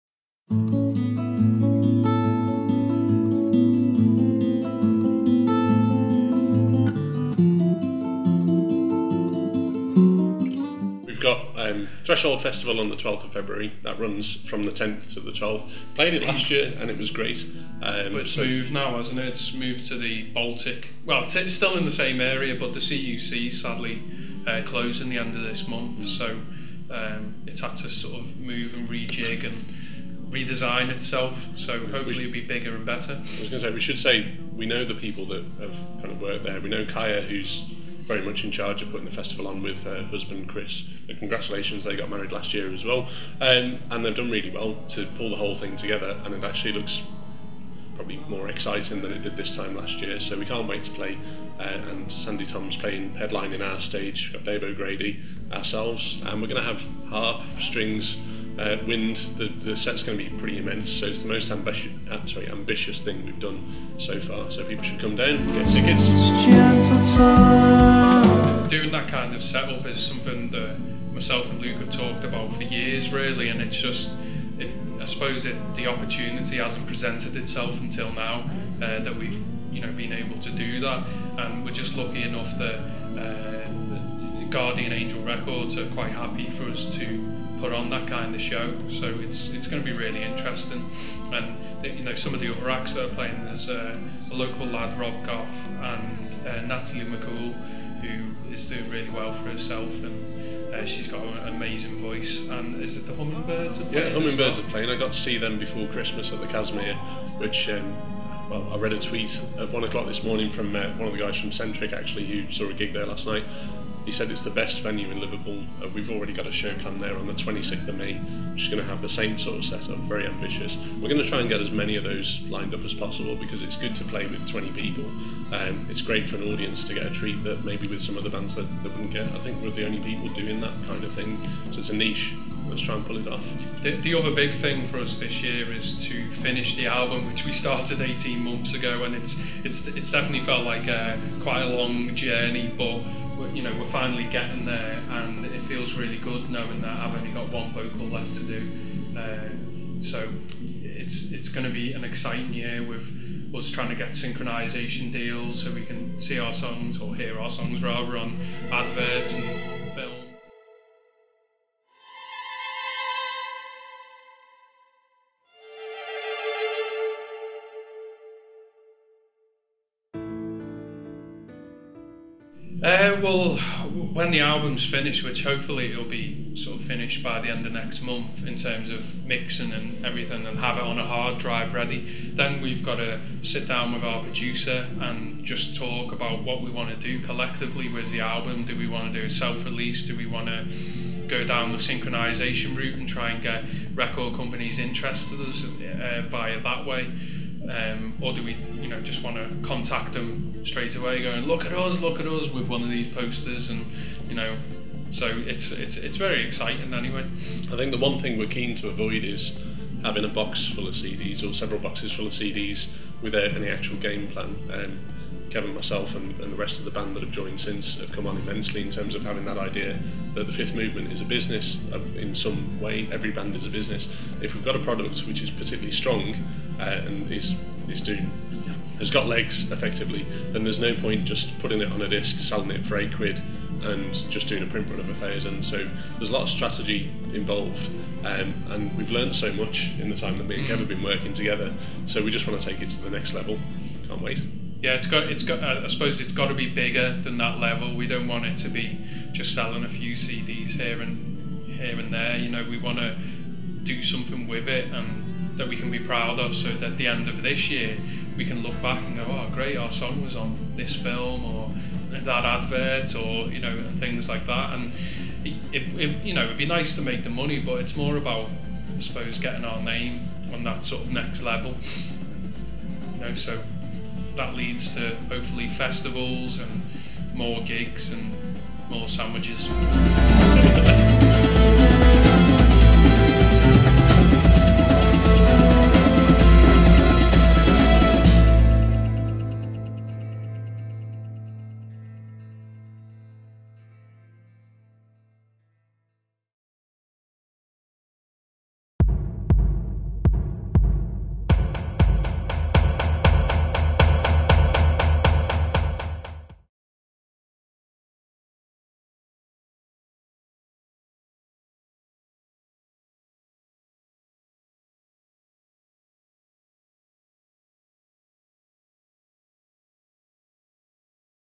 Liverpool Live TV talk to The Fifth Movement about Threshold 2012 and 2012 in general.
Jamie McAleny from Liverpool Live TV talks to Luke Moore and Kevin Critchley from The Fifth Movement about their plans for 2012. Their upcoming gigs. Their Threshold Festival 2012 appearance, and their plans for the NEW album. coming soon.
This video contains Threshold Festival Trailers.